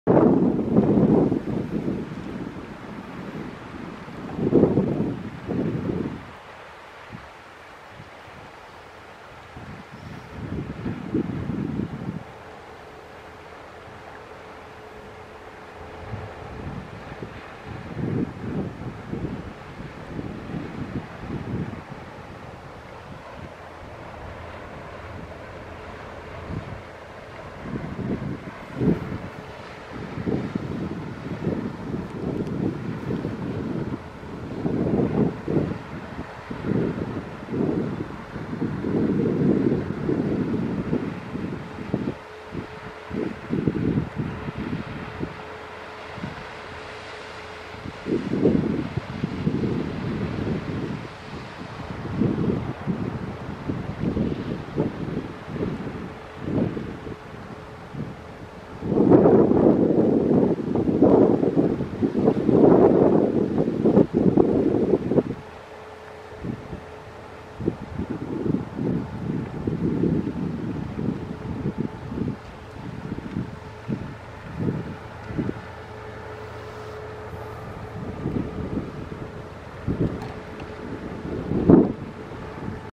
koppelverband Impuls
Kvb Impuls 🤜⚓🤛 in de opvaart op de Waal bij Ooij